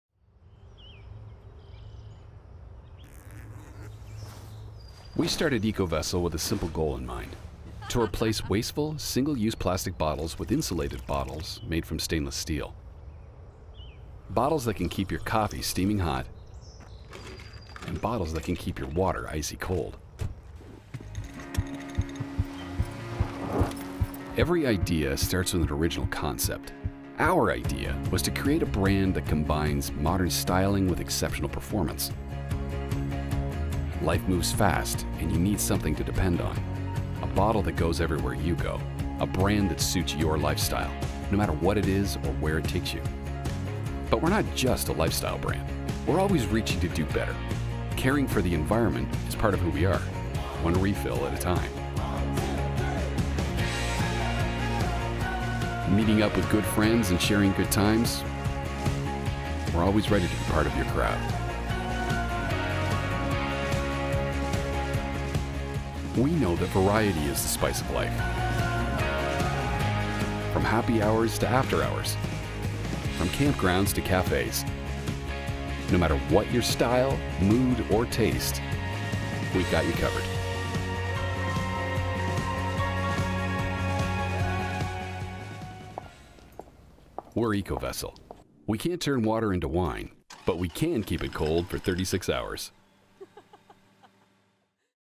ECOVESSEL Brand Video
Premium insulated stainless steel water bottles, tumblers, coffee mugs, stemless wine glasses for coffee, beer, water, and wine. Our BPA free bottles include our signature TriMax Insulation, are made from food grade 18/8 steel, include strainers for tea and fruit infusion, and come with a 100-year Warranty.